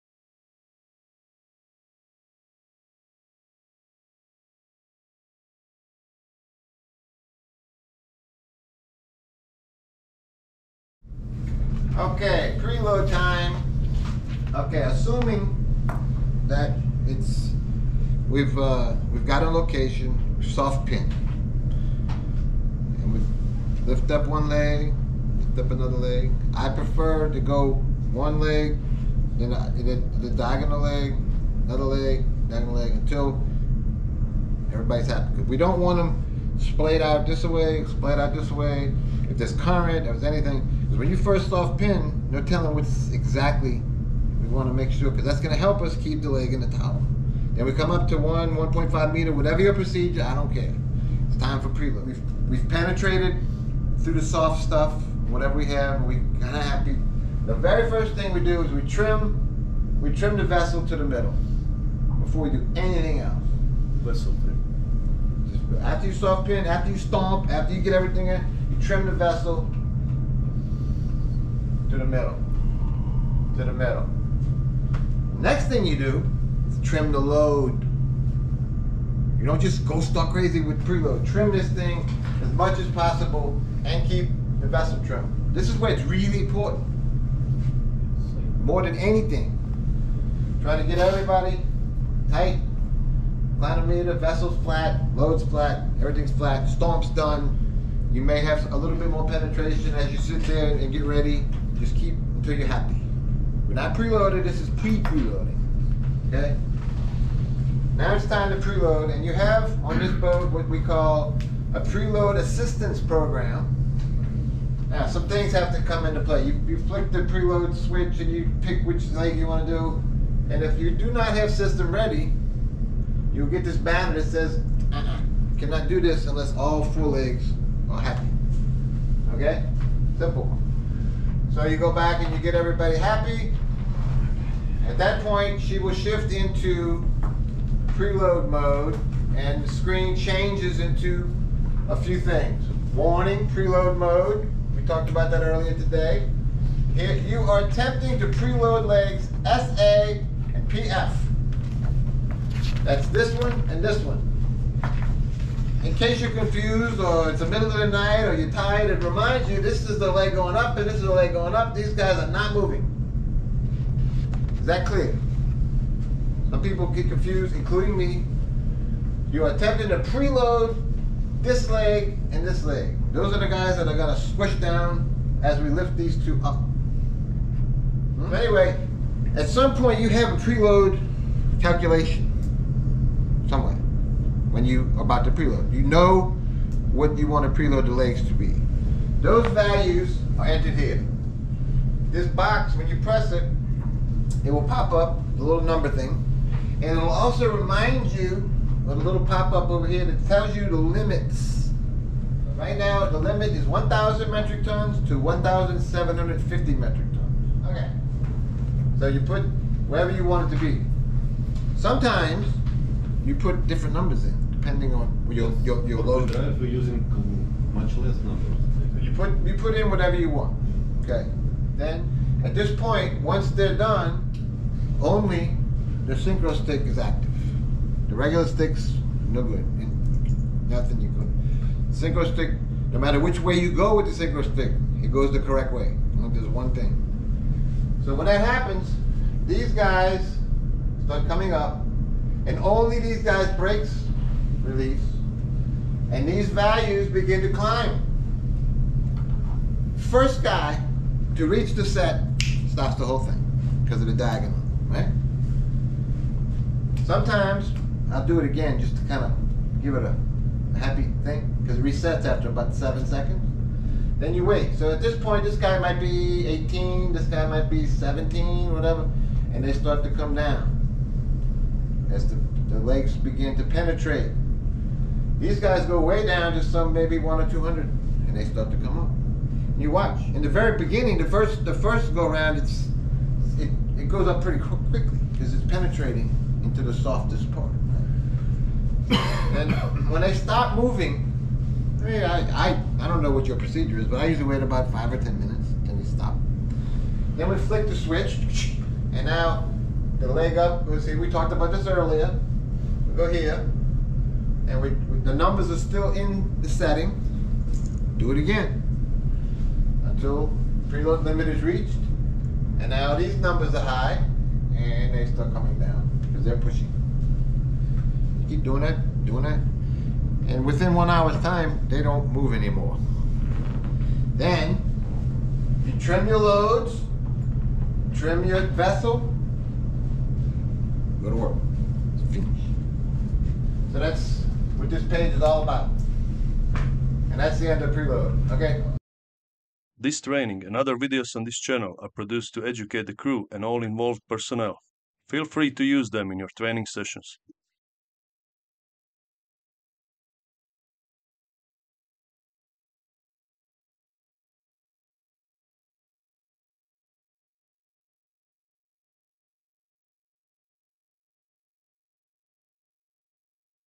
JACKING SYSTEM - Lesson 16 - PRELOAD SCREEN
PRELOAD OPERATION, explained by the system designer. Essential training for every CHIEF ENGINEER and senior personnel working on a jackup, as well as the rest of the crew who are interested in how the system works.